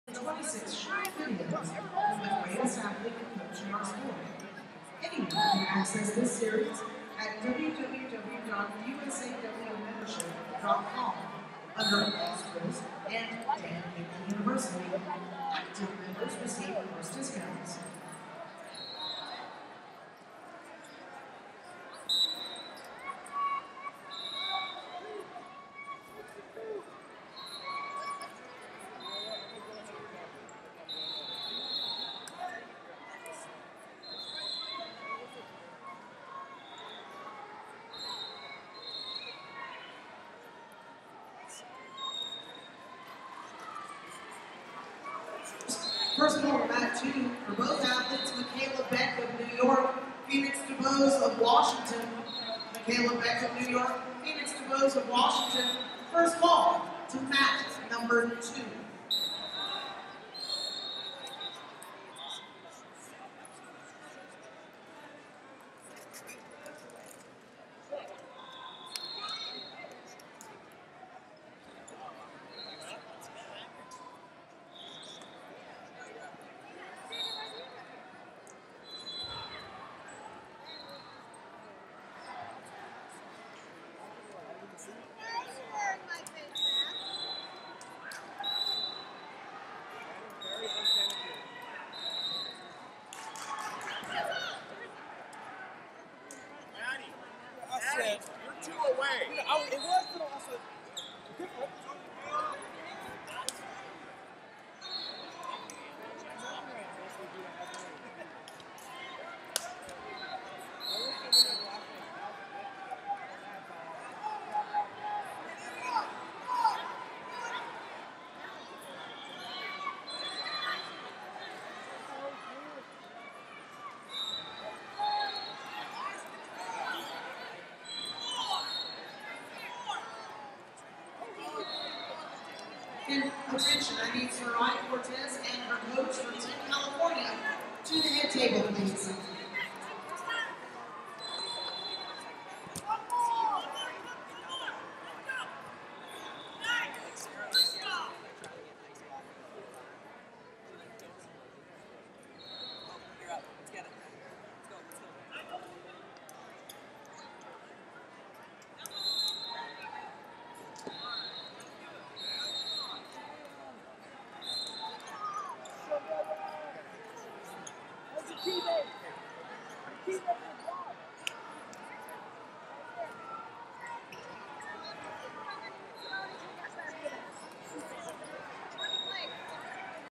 59 Kg Semifinal - Michaela Beck, NY Vs Phoenix Dubose, WA
FloSports/2021 Women's National Championships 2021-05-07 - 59 Kg Semifinal - Michaela Beck, NY Vs Phoenix Dubose, WA